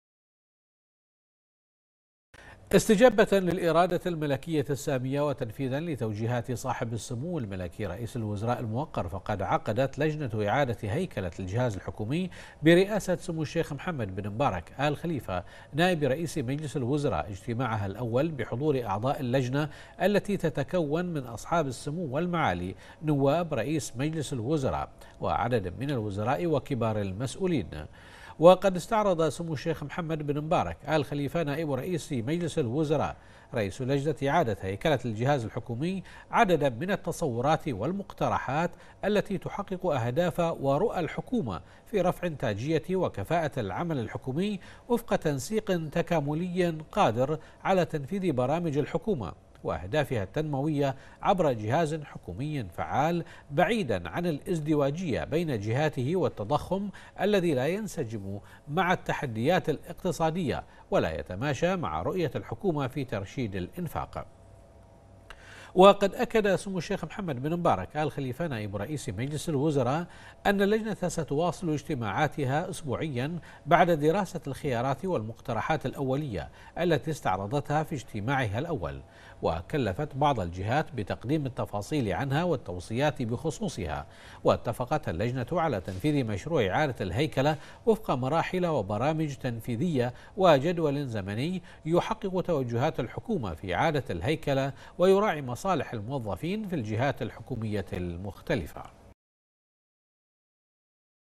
البحرين: سمو نائب رئيس مجلس الوزراء يرأس إجتماع لجنة إعادة هيكلة الجهاز الحكومي
المنامة في 21 مارس / بنا / استجابة للإرادة الملكية السامية، وتنفيذاً لتوجيهات صاحب السمو الملكي رئيس الوزراء الموقر فقد عقدت لجنة إعادة هيكلة الجهاز الحكومي برئاسة سمو الشيخ محمد بن مبارك آل خليفة نائب رئيس مجلس الوزراء اجتماعها الأول بحضور أعضاء اللجنة التي تتكون من أصحاب السمو والمعالي نواب رئيس مجلس الوزراء وعدد من الوزراء وكبار المسئولين.